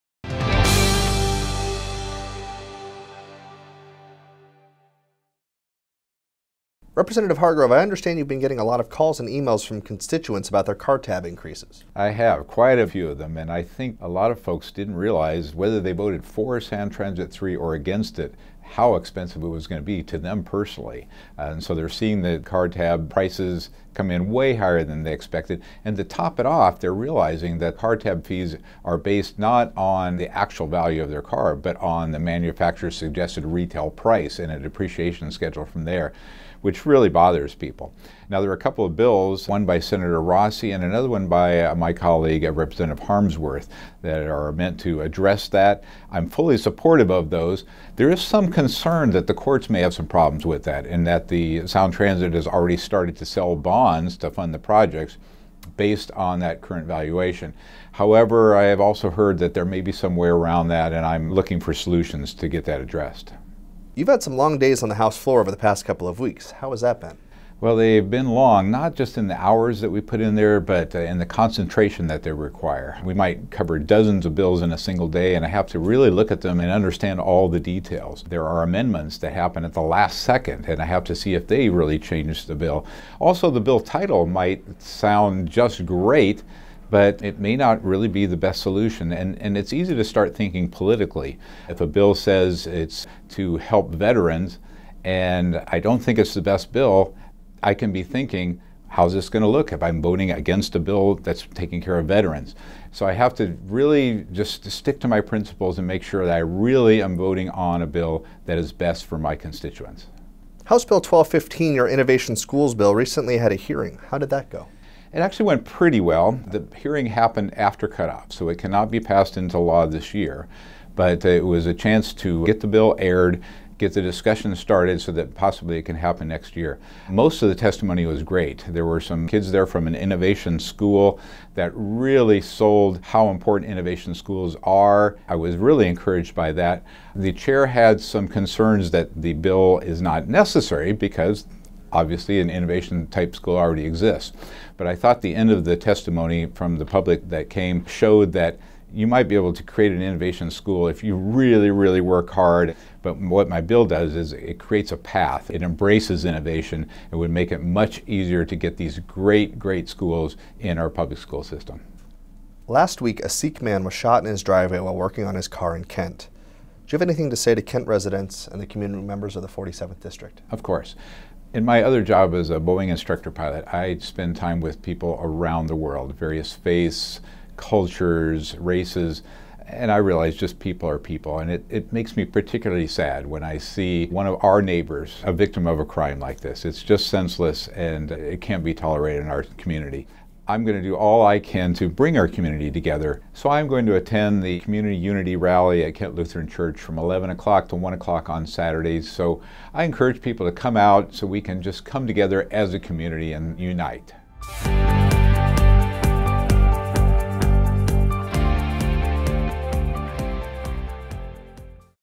Rep. Mark Hargrove’s legislative video update | March 9, 2017
In his latest video update, Rep. Hargrove provides his thoughts on last week’s senseless shooting of a Sikh man in Kent, and invites community members to join him at a unity rally. He also discusses the car-tab sticker shock many are feeling as a result of the passage of Sound Transit 3, as well as the public hearing his innovation schools bill received.